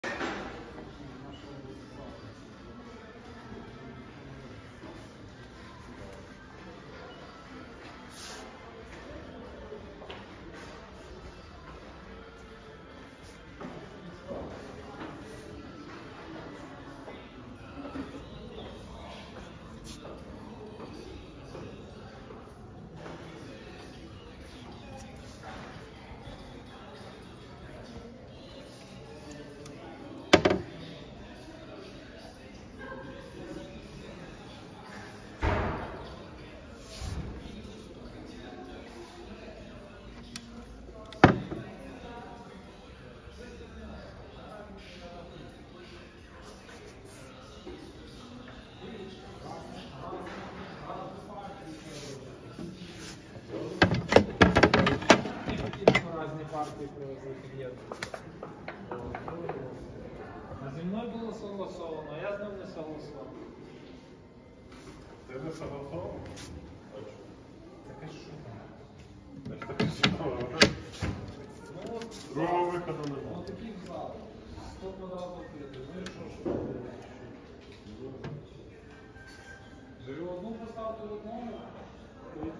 How to remove the cover of the Villeroy Boch Venticello wall-hung toilet 4611RS01
How to remove the cover of the villeroy Boch Venticello toilet 4611RS01 VENTICELLO
4611RS01 VENTICELLO унітаз підвісний + кришка SoftClose QuickRelease, білий